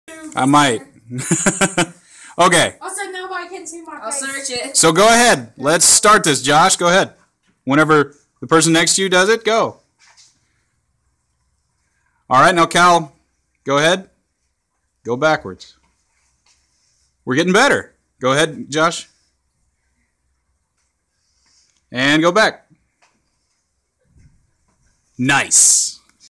Physical Science does the wave
The Piarist School physical science class demonstrates a transverse wave by doing the wave.